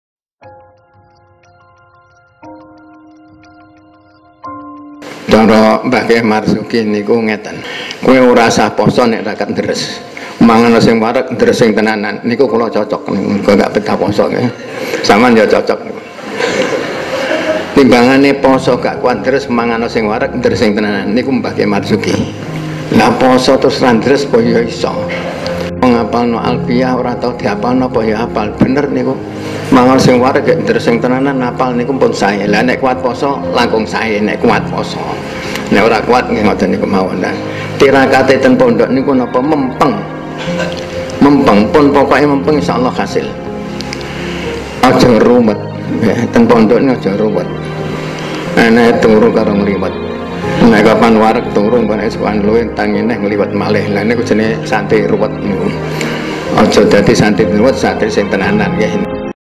KH. Anwar Manshur - Jangan menjadi santri yang nakal.
KH. Anwar Manshur :"kalau tidak kuat puasa, tirakatnya santri dipesantren itu mempeng, rajin belajar.  daripada puasa tapi akhirnya malah gak kuat dibuat belajar, mending makan yang kenyang, belajar yang rajin (mempeng)"